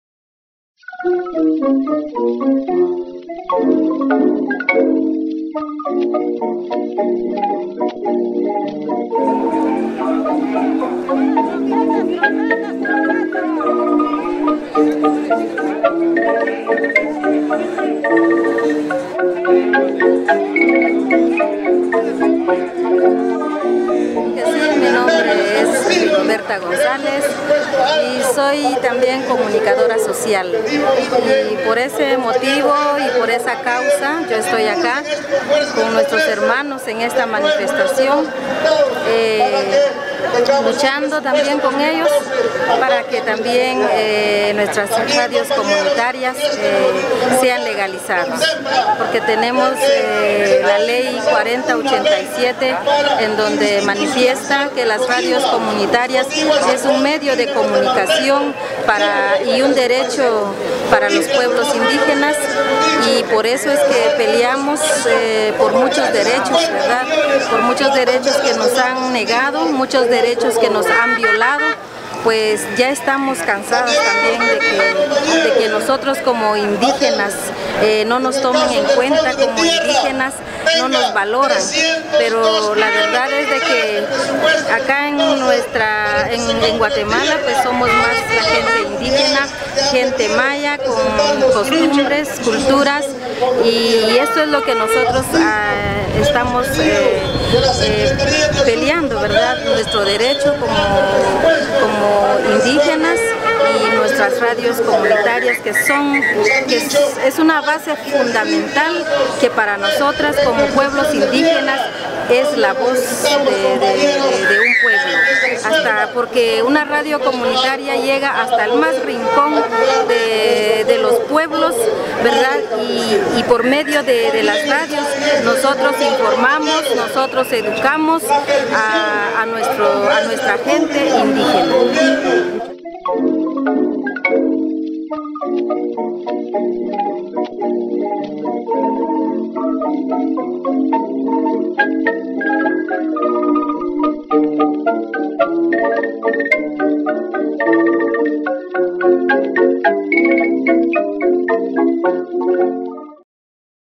Marcha 12 Octubre 2011 entrevista con Rigoberta Gonzales
Marcha del 12 de Octubre 2011. Entrevista con la compañera Rigoberta Gonzáles de las radios comunitarias en Guatemala.
Protest on 12th October 2011. Interview with Rigoberta Gonzáles from the Guatemala Community Radio Project.